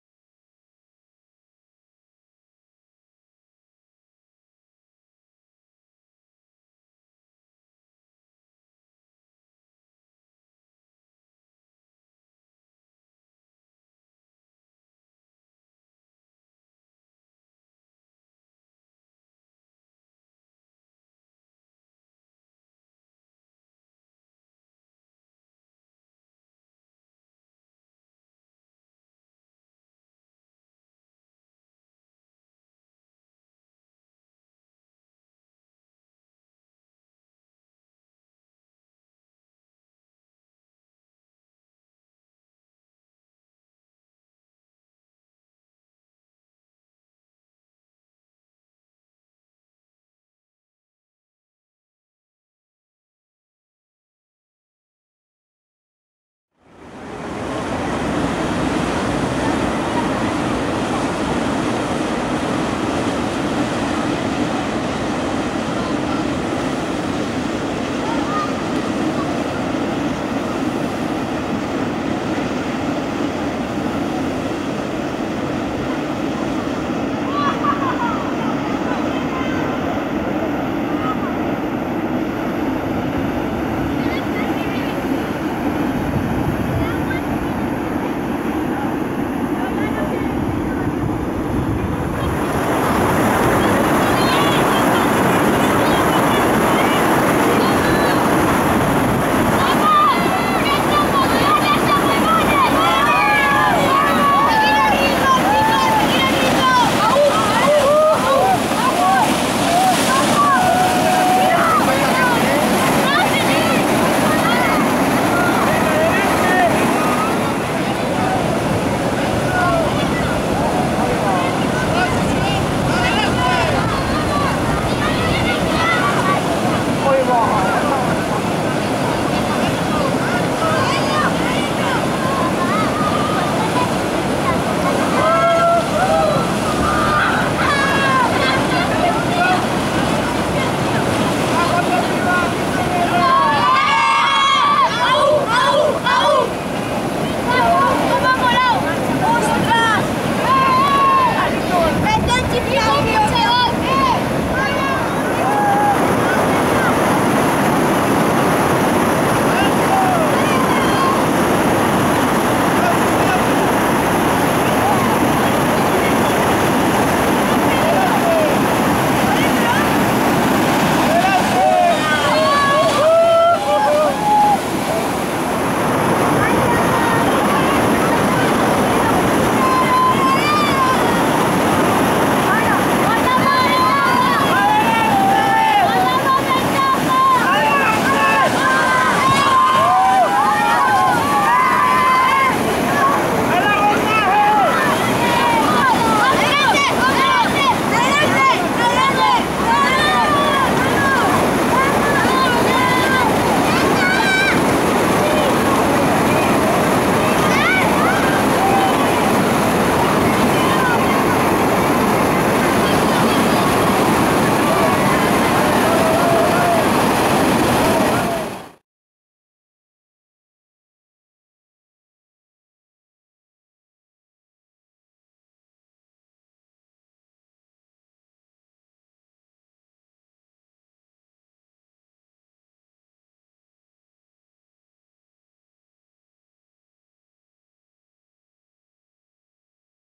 AGUAS BRAVAS, EN EL PARQUE DEL AGUA DE ZARAGOZA (v.1)
La Torre del Agua y el Canal de Aguas Bravas en el Parque del Agua (Zaragoza).